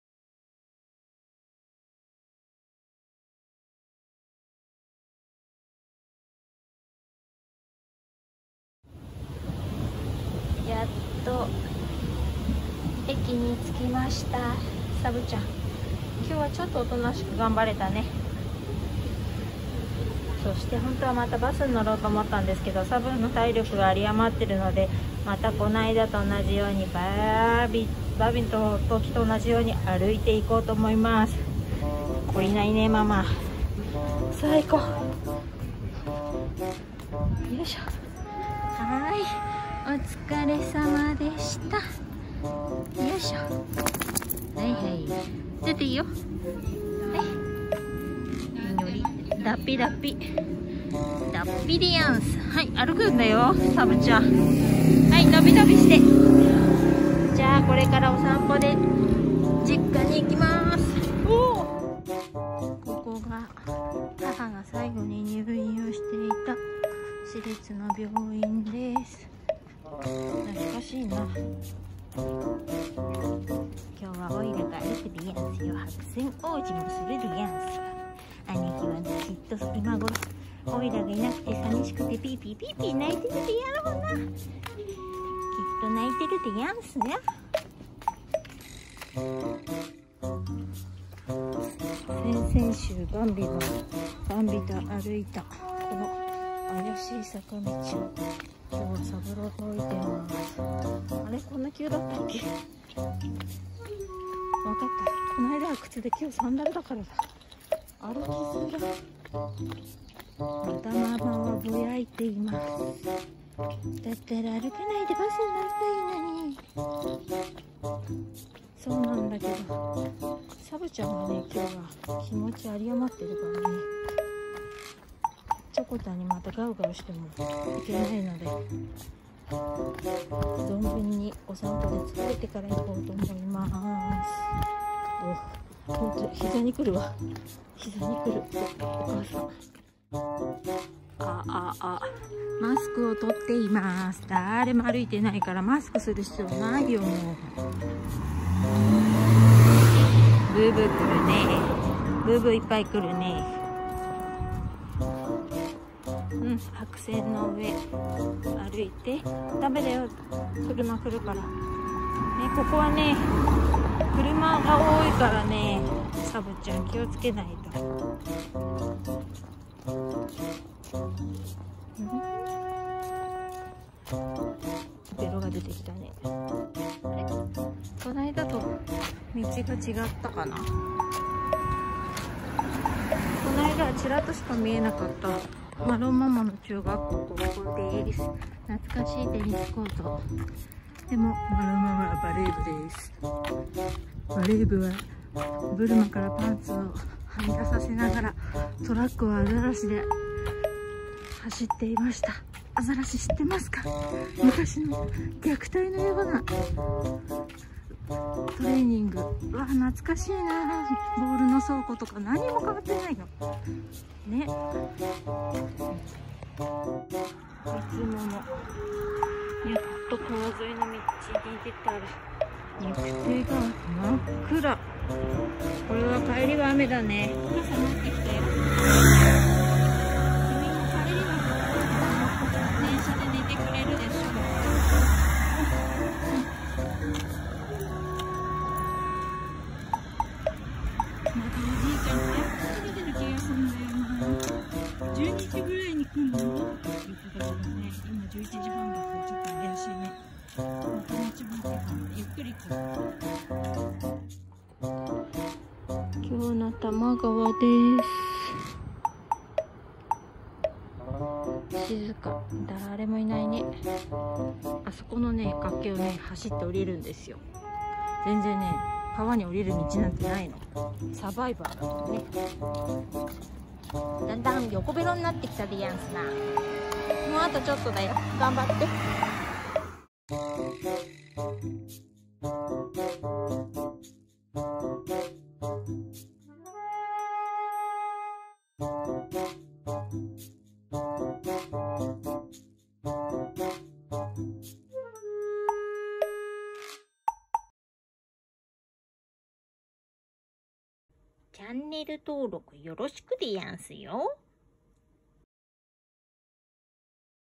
サブと田舎さんぽ
さいごにちょこたろうに逢いに行く日のさぶさんぽ。

Marroppy's！